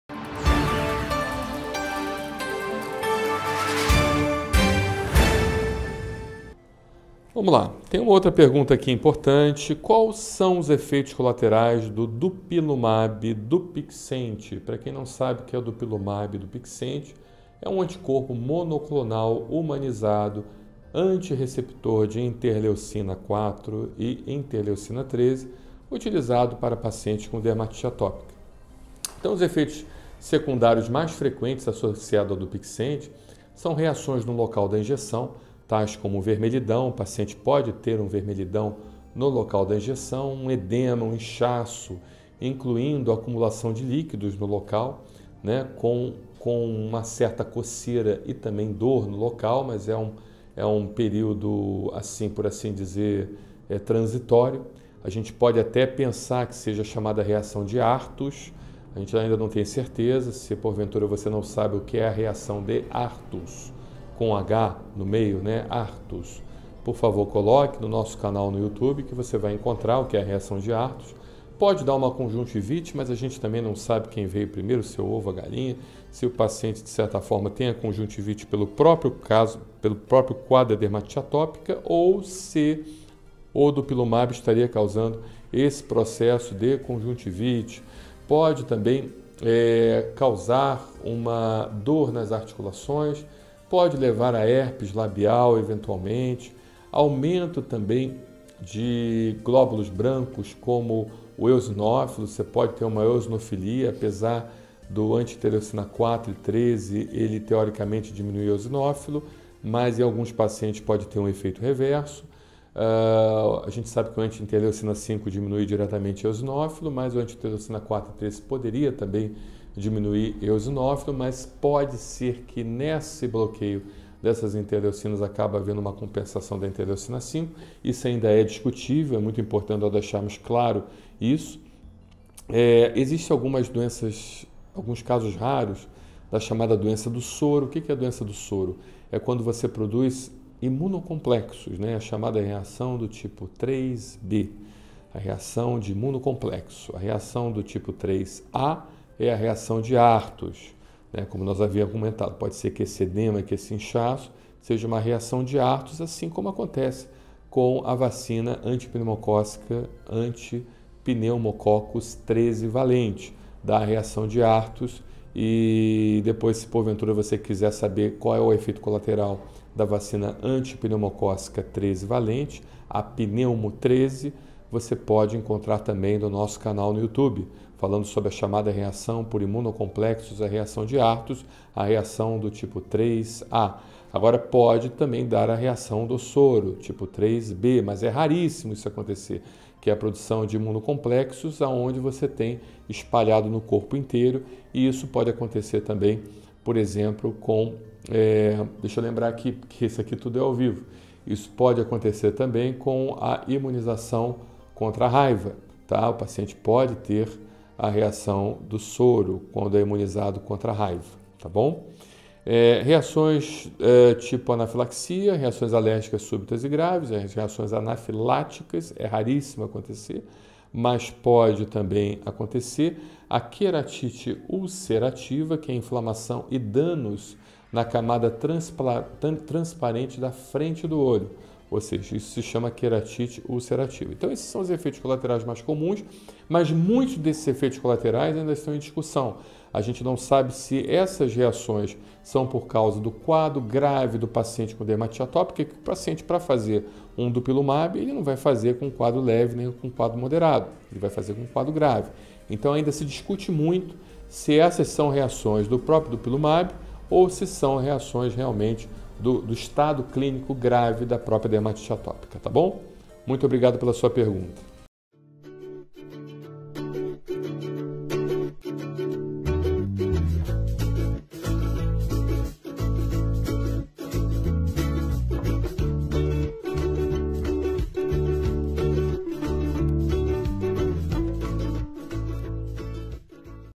DUPILUMABE DUPIXENT | Quais são os EFEITOS COLATERAIS?
Pergunta - Quais são os efeitos colaterais do Dupilumabe Dupixent?

Clínicas do Brasil Sem Alergia

Frases para efeitos de dinamização do vídeo:
Dupilumabe é um agente imunorregulador direcionado que inibe de maneira seletiva e simultânea a sinalização da IL-4 e IL-13, bloqueando o componente compartilhado obrigatório do complexo do receptor de IL-4/IL-13. Destina-se a inibir os principais fatores desencadeantes da doença para obter benefício clínico sem os efeitos colaterais comumente observados com os imunossupressores sistêmicos não seletivos existentes. O dupilumabe demonstrou benefícios clinicamente significativos, combinados com um perfil de segurança favorável, sem a toxicidade e os efeitos colaterais comumente observados com os imunossupressores sistêmicos não seletivos. Foi a primeira terapia biológica aprovada para pacientes adultos e adolescentes com DA e agora também foi demonstrado um perfil de benefício-risco positivo no tratamento da dermatite atópica em crianças =6 anos de idade.
O DUPIXENT é aprovado no momento para dermatite atópica, asma e rinossinusite crônica com pólipo nasal.
A ampliação de uso se refere somente à indicação de dermatite atópica, cujo texto na bula passa a vigorar com a seguinte redação.
“Crianças de 6 a 11 anos de idade.
DUPIXENT é indicado para o tratamento de crianças de 6 a 11 anos de idade com dermatite atópica grave cuja doença não é adequadamente controlada com tratamentos tópicos ou quando estes tratamentos não são aconselhados. DUPIXENT pode ser utilizado com ou sem corticosteroide tópico.’’
Denomina-se efeito colateral como um efeito diferente daquele considerado como principal por um fármaco. Esse termo deve ser distinguido de efeito adverso, que se refere a um efeito colateral indesejado, pois um fármaco pode causar outros efeitos potencialmente benéficos além do principal.